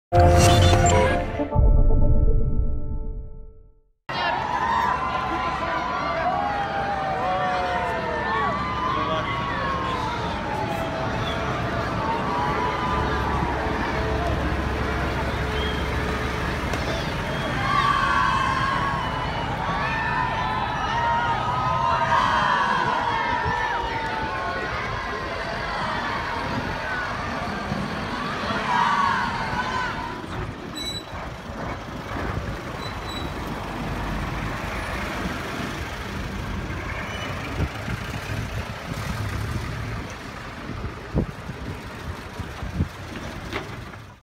Начали отмечать День победы
В донской столице стартовали торжества по случаю 71-й годовщины Победы.